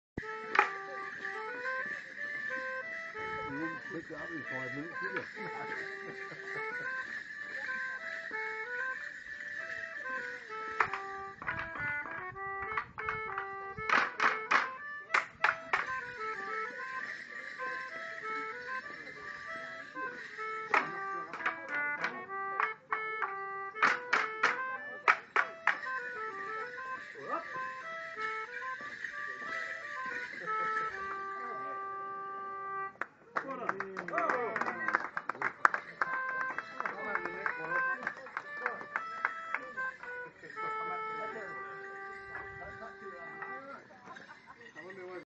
The Woodchurch Morris Men perform outside the Shepherd & Crook Inn, Burmarsh
Without local assistance!!